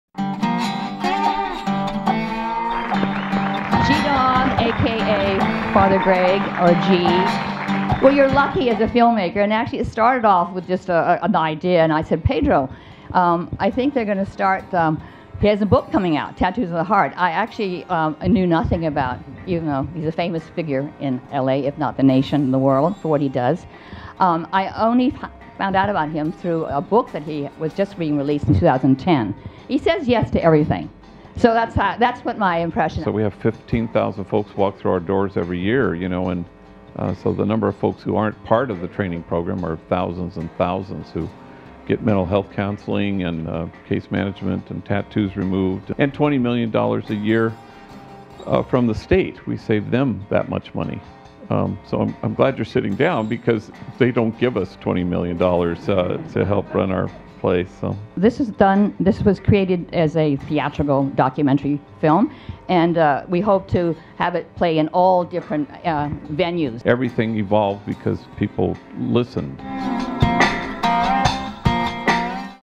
2012 LA FILM FEST - G-Dog Q&A
Freida Mock talks about her experience uncovering Father Greggory Boyle and his story.